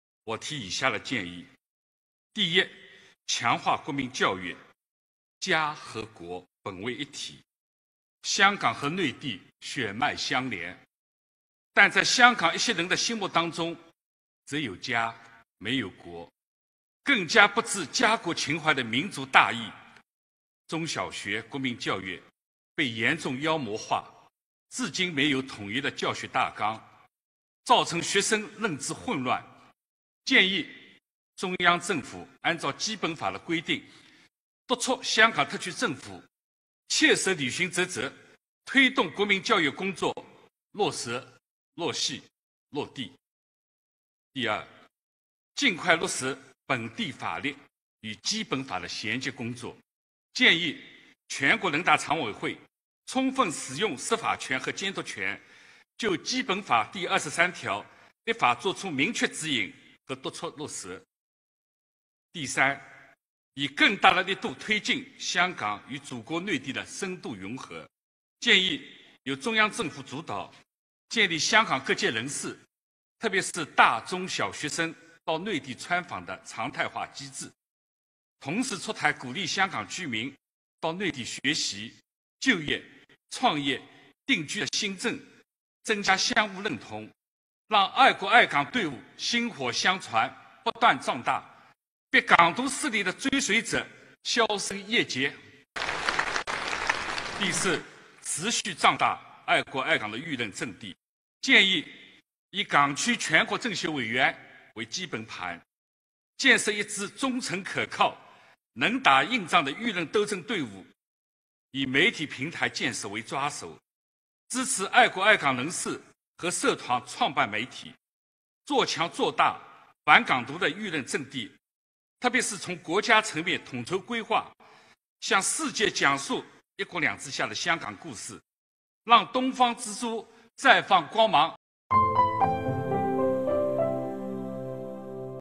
屠海鸣：建议督促香港推动国民教育落细落地
全国政协十三届三次会议第二次全体会议5月24日上午9时在人民大会堂举行，委员进行大会发言。全国政协委员、香港新时代发展智库主席屠海鸣说，要坚定“一国两制”制度自信，该坚持的一定要坚持，该做工作的一定要耐心细致地做，彻底铲除“港独”势力的生存土壤。
        为此，他建议，强化国民教育。中央政府按照基本法规定，督促香港特区政府切实履行职责，推动国民教育工作落实落细落地。
        尽快落实本地法律与基本法衔接工作。屠海鸣建议全国人大常委会充分使用释法权和监督权，就基本法“二十三条”立法作出明确指引和督促落实。
        以更大力度推进香港与内地深度融合。屠海鸣建议，由中央政府主导，建立香港各界人士、特别是大中小学生到内地参访的常态化机制。同时，持续出台鼓励香港居民到内地学习、就业、创业、定居的新政，增进相互认同，让爱国爱港队伍薪火相传，不断壮大，逼“港独”势力的追随者销声匿迹。
        持续壮大爱国爱港舆论阵地。屠海鸣建议，以港区全国政协委员为“基本盘”，建设一支忠诚可靠、能打硬仗的舆论斗争队伍；以媒体平台建设为抓手，支持爱国爱港人士和社团创办媒体，做强做大反“港独”的舆论阵地；特别是从国家层面统筹谋划，向世界讲述“一国两制”下的香港故事，让“东方之珠”再放光芒。